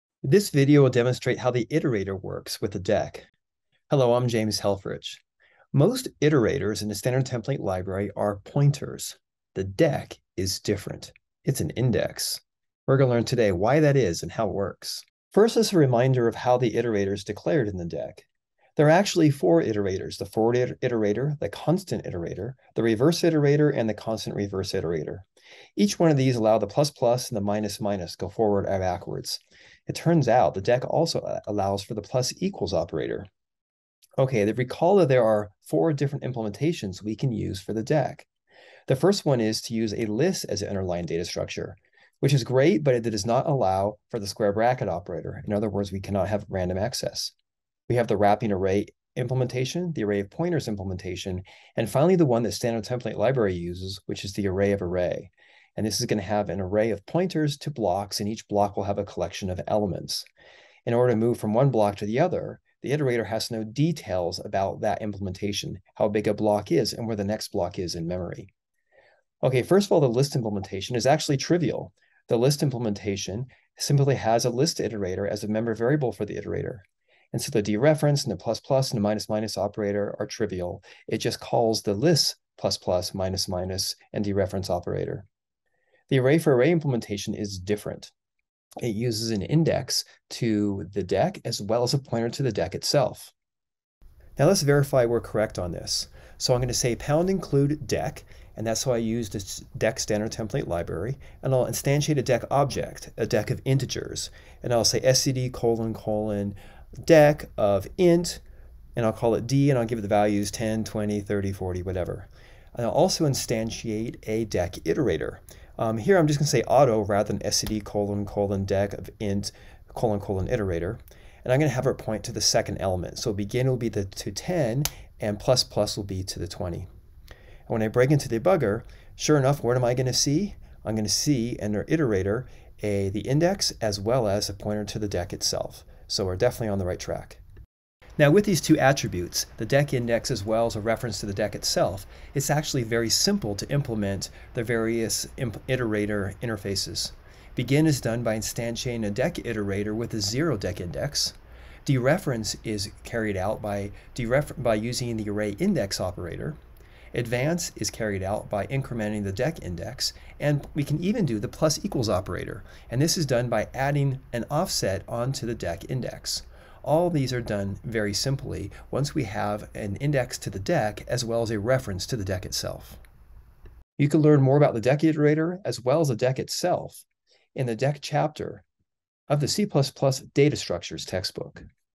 C++ Deque - How to implement a deque iterator
The C++ standard template library implements the iterator for the deque in a different way that it does so with other containers. This video will demonstrate how this works.

You can learn more about the deque and other data structures in C++ Data Structures by James Helfrich

0:00 Introduction
0:15 Iterator Syntax
0:33 Four different implementation options
1:08 Iterator for the list implementation
1:25 Iterator for the array implementations
1:34 Demonstration of the STL deque iterator
2:17 Pseudocode of the main iterator methods